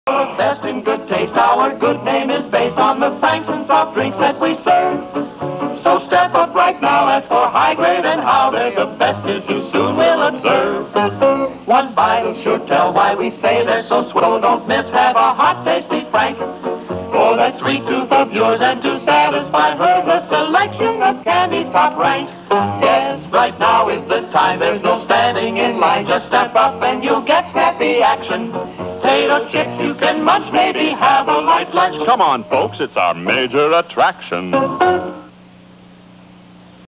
Vintage Drive-In Intermission video 2
Hygrade's hot dogs drive-in intermission video...delicious!